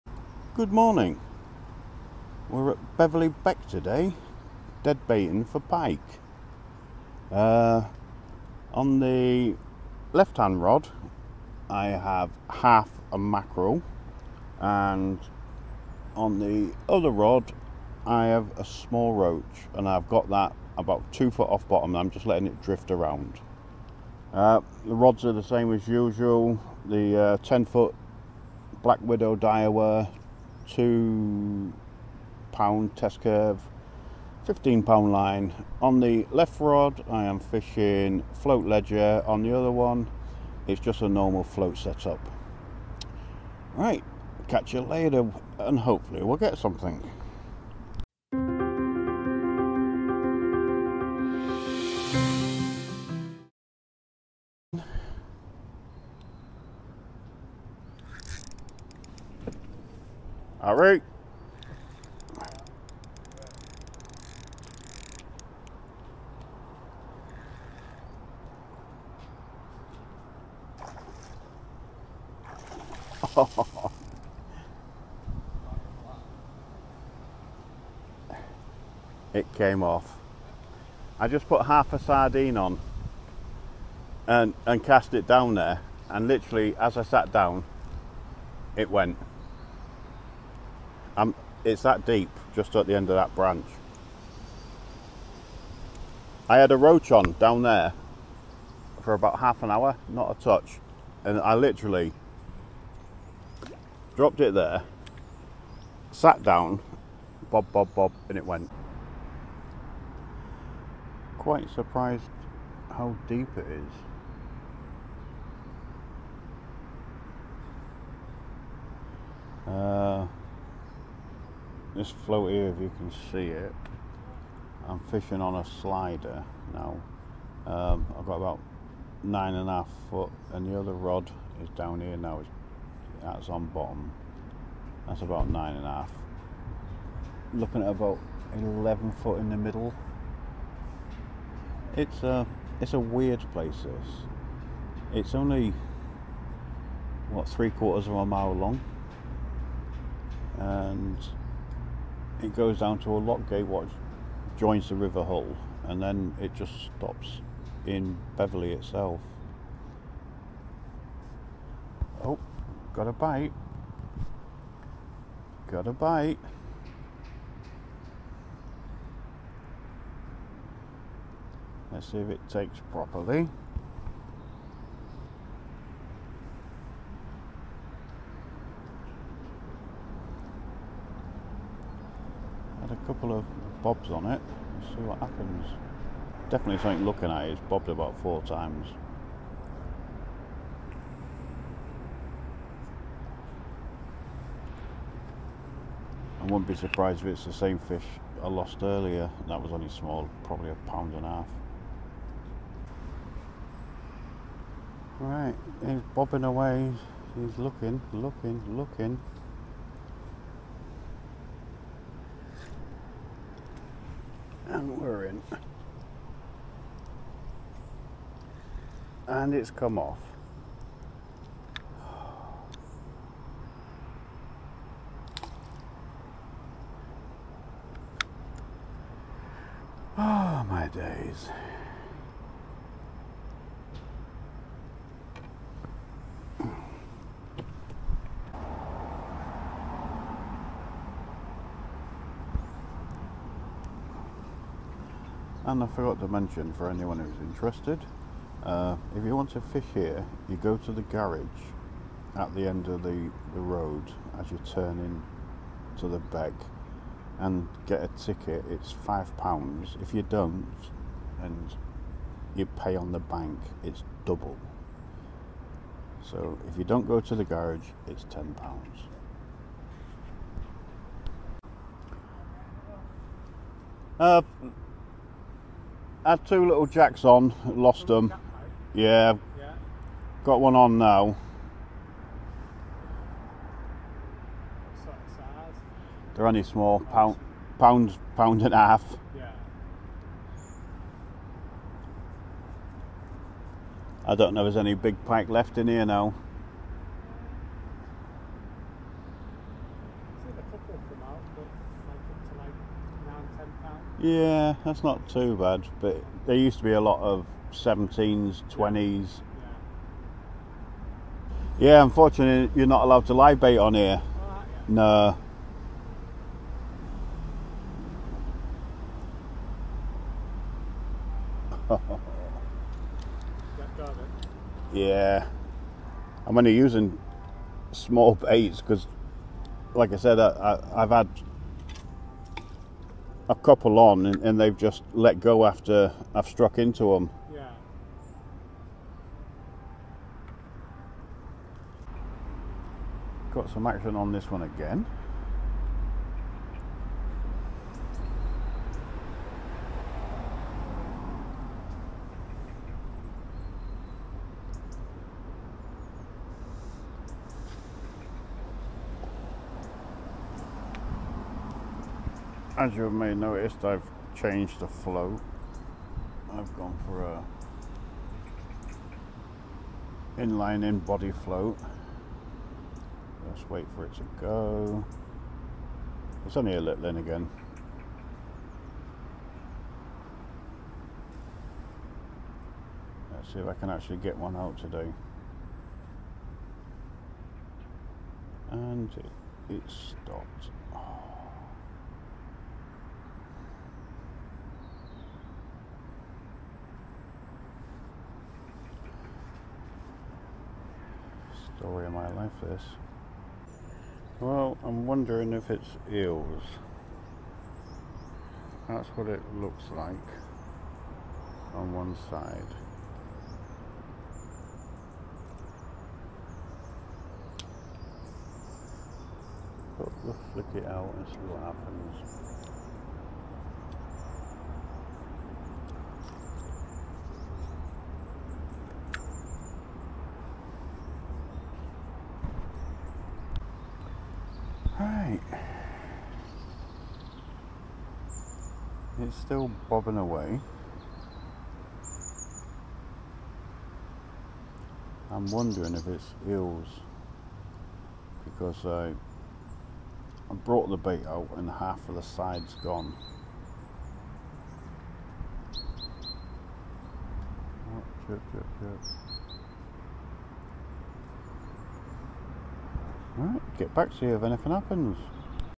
Beverley Beck deadbaiting, one of those days! oct 2023.
In this video, I'm sharing one of those days where everything goes wrong...literally! I'm fishing Beverley Beck near Hull,  and in this video I'm deadbait fishing for Pike on one of those days where the fish just don't want to play. It's not the most glamorous of videos, but it's one of those videos that you just have to see.